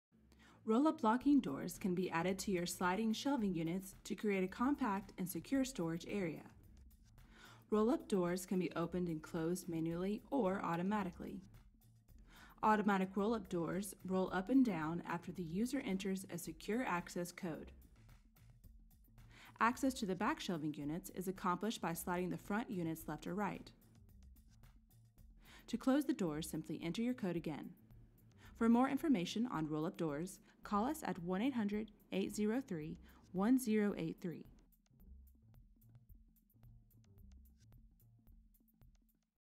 Roll Down Shelving Doors to Secure Storage of Files Supplies Inventory Parts
Roll down shelving doors will fit on just about any type of shelving to secure storage of files, supplies, inventory, and parts. Roll down shelving doors are better than traditional hinged doors that swing out into the aisle way.  These shelving doors cover more shelving units requiring less keys to keep up with and they don’t create obstructions by swinging out into the aisle way. Another cool feature is the ability to access stored items if boxes are stacked in front of the doors because the doors roll up and down instead of swinging out.